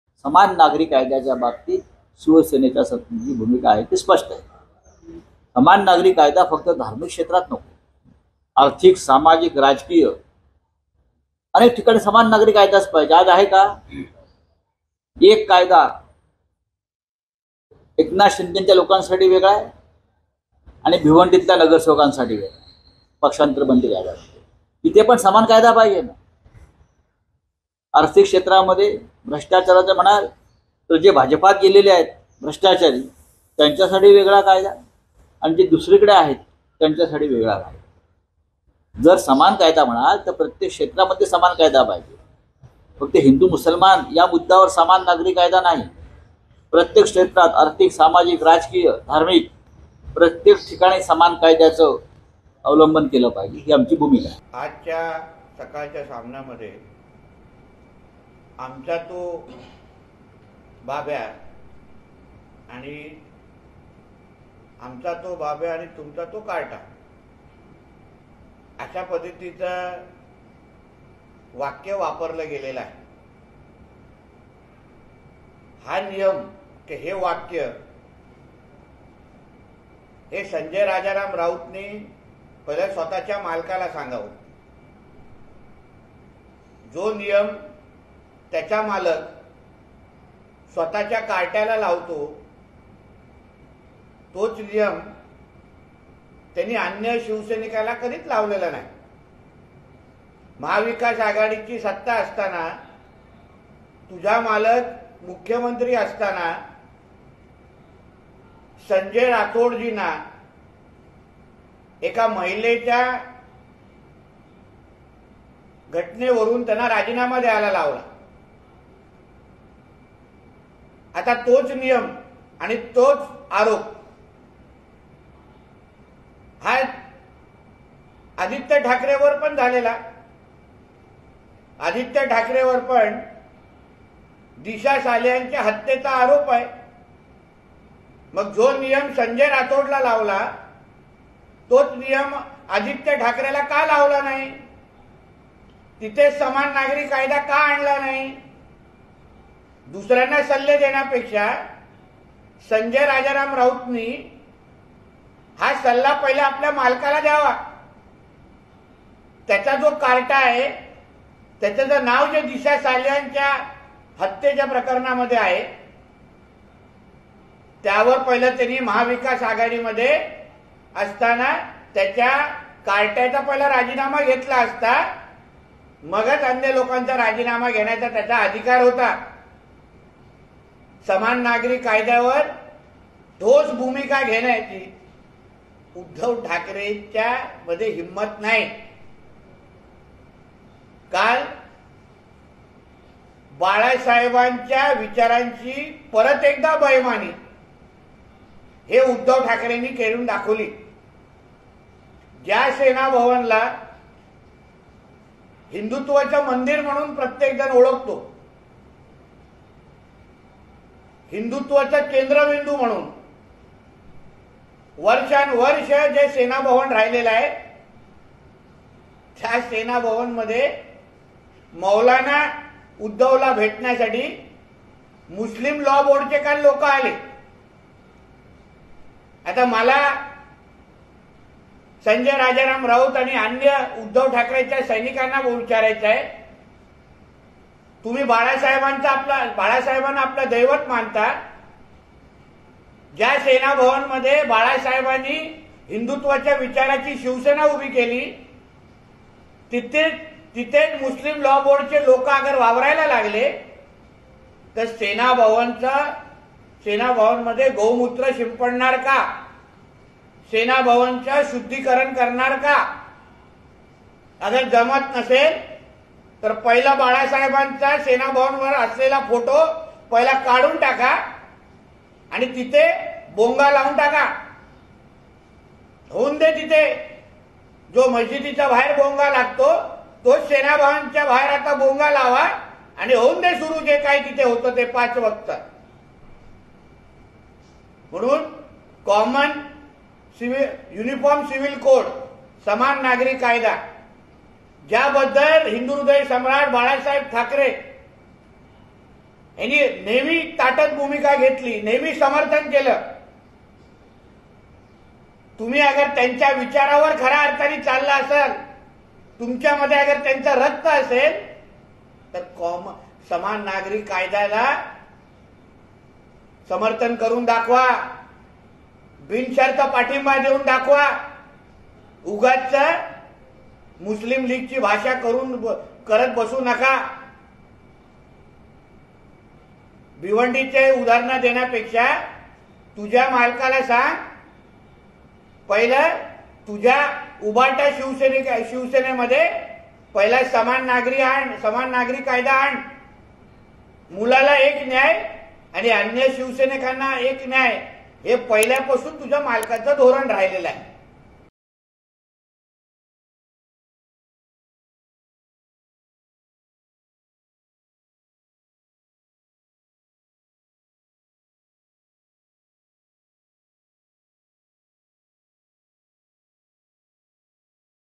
uniformcivilcode : राऊतांचे आरोप, राणेंचे सडेतोड प्रत्युत्तर : LetsUpp Marathi
समान नागरी कायद्याच्या मुद्द्यावरूनही संजय राऊत यांनी भाजपवर टीका केली. त्याला नितेश राणे यांनी सडेतोड प्रत्युत्तर दिले.